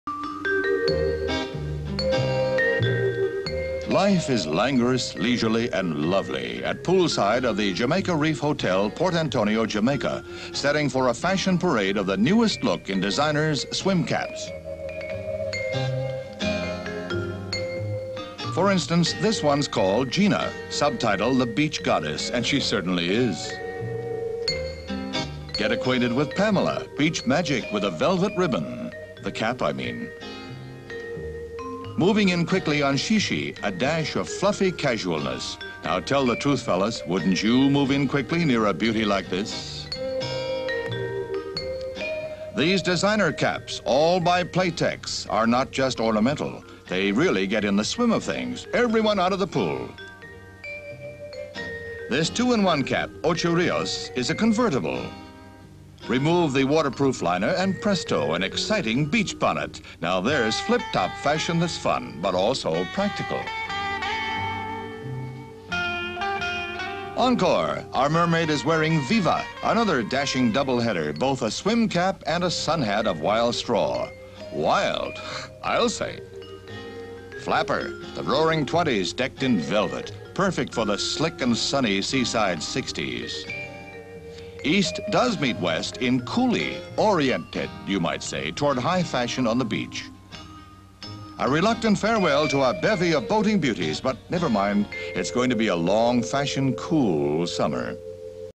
Sexy Summer Swim Cap Fashions 1966 Newsreel PublicDomainFootage.com
1:38 Newsreel.  Beautiful models show off their sexy 1966 swim cap styles for the camera.